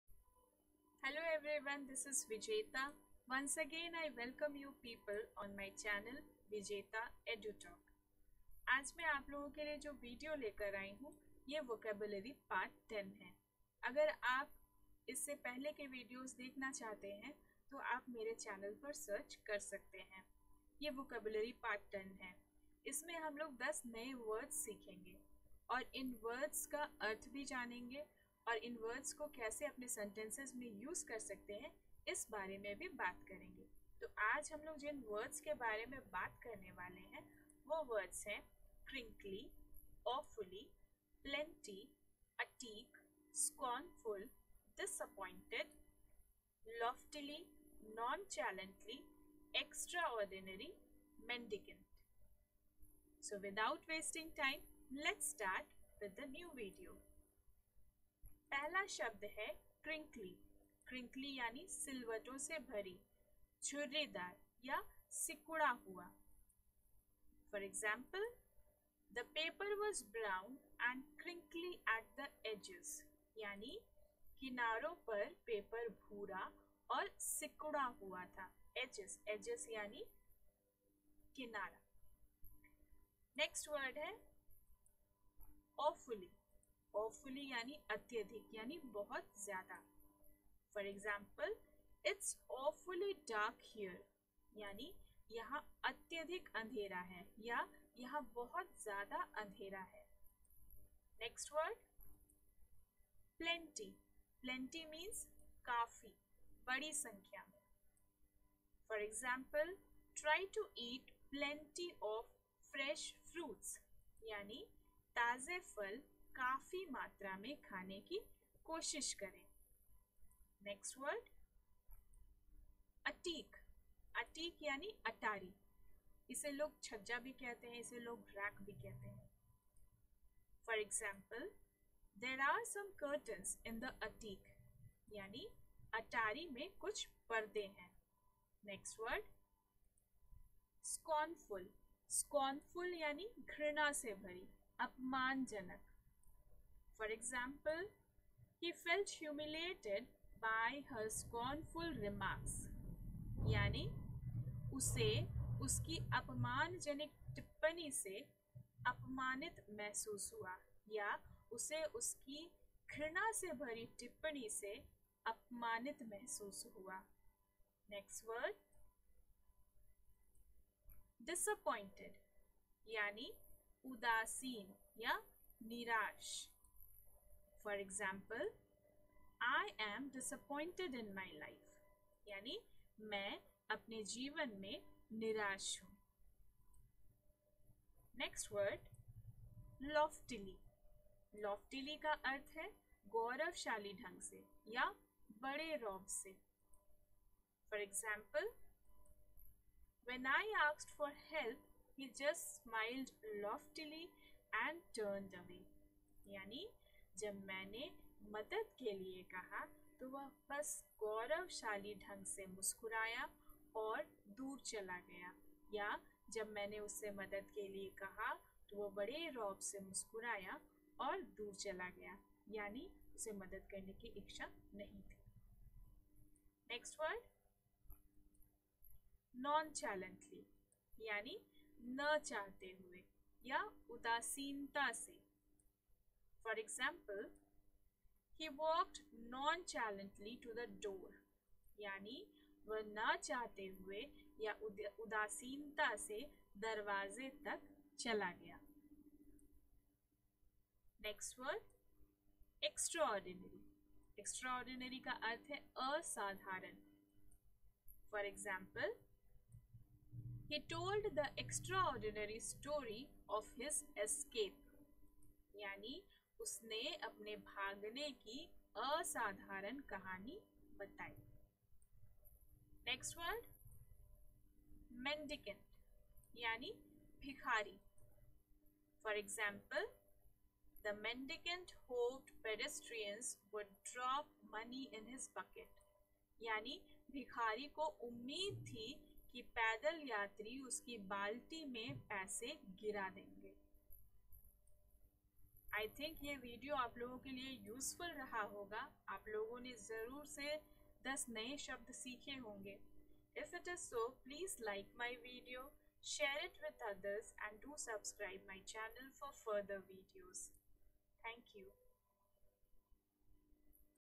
VOCABULARY I PART 10 I WITH MEANING IN HINDI I TO SPEAK IN ENGLISH CONFIDENTLY I
This video enhances your word stock, makes you learn a few new words, their meanings and uses in sentences as well.